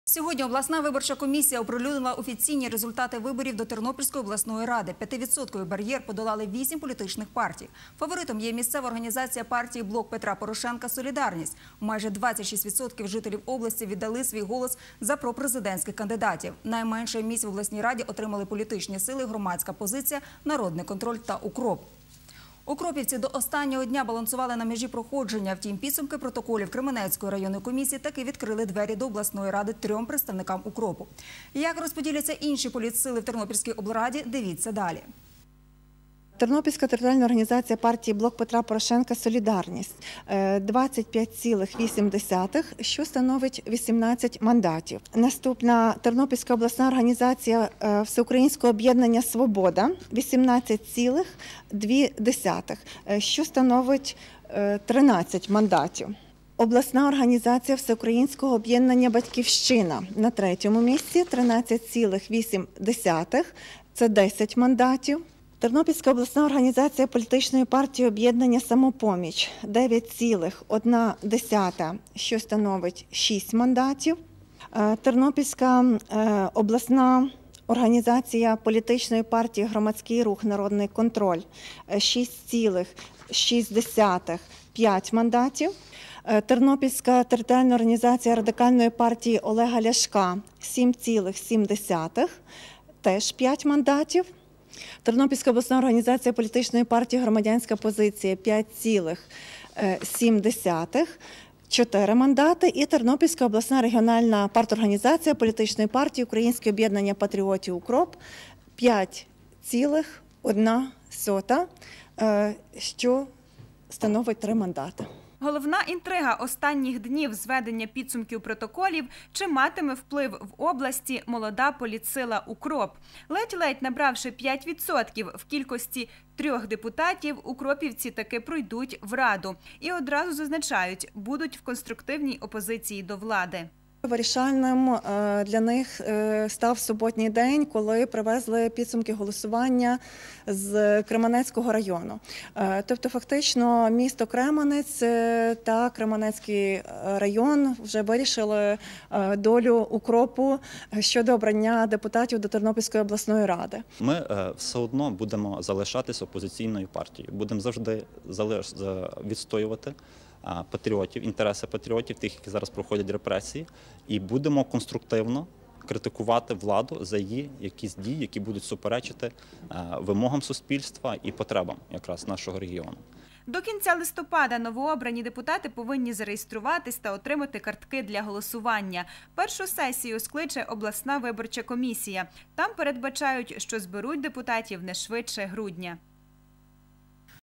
Підбито підсумки виборів до Тернопільської облради
обласна виборча комісія оприлюднила офіційні результати виборів до Тернопільської обласної ради. 5% - бар’єр подолали вісім політичних партій. Фаворитом  є місцева організації партії Блок Петра Порошенка «Солідарність»,  майже 26 % жителів області віддали свій голос за пропрезидентських кандидатів. Найменше місць в обласній раді  отримають політичні сили Громадянська позиція, Народний контроль та УКРОП. Укропівці до останнього дня балансували на межі проходження, втім підсумки протоколів Кременецької районної комісії таки відкрили двері до обласної ради трьом представникам УКРОПу.